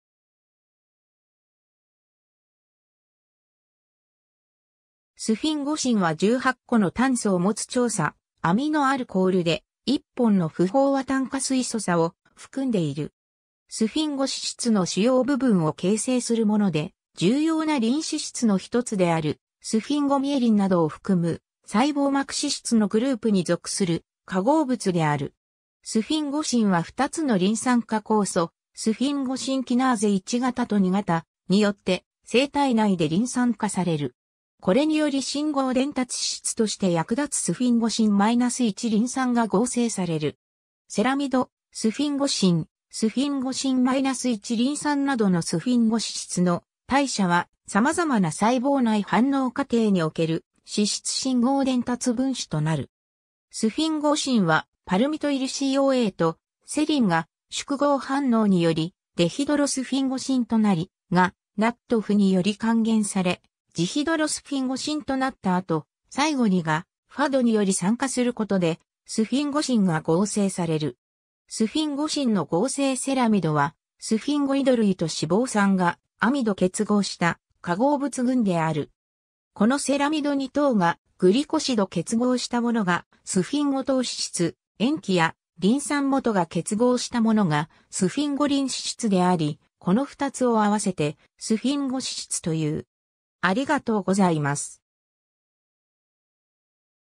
スフィンゴシン
スフィンゴシン（2-アミノ-4-オクタデセン-1,3-ジオール）は18個の炭素を持つ長鎖アミノアルコールで、1本の不飽和炭化水素鎖を含んでいる。
スフィンゴ脂質の主要部分を形成するもので、重要なリン脂質のひとつであるスフィンゴミエリンなどを含む細胞膜脂質のグループに属する化合物である。
 スフィンゴシンはふたつのリン酸化酵素、スフィンゴシンキナーゼ1型と2型、によって生体内でリン酸化される。
これにより信号伝達脂質として役立つスフィンゴシン-1-リン酸が合成される。
 セラミド、スフィンゴシン、スフィンゴシン-1-リン酸などのスフィンゴ脂質の代謝は、様々な細胞内反応過程における脂質信号伝達分子となる。
 スフィンゴシンは、パルミトイルCoA（A）とセリン（B）が縮合反応によりデヒドロスフィンゴシン（C）となり、（C）がNADPHにより還元されジヒドロスフィンゴシン（D）となった後、最後に（D）がFADにより酸化することで、スフィンゴシン（E）が合成される。
 スフィンゴシンの合成 セラミドは、スフィンゴイド類（スフィンゴシンはスフィンゴイド類の一種である）と脂肪酸がアミド結合した化合物群である。
このセラミドに糖がグリコシド結合したものがスフィンゴ糖脂質、塩基やリン酸基が結合したものがスフィンゴリン脂質であり、この二つを合わせてスフィンゴ脂質という。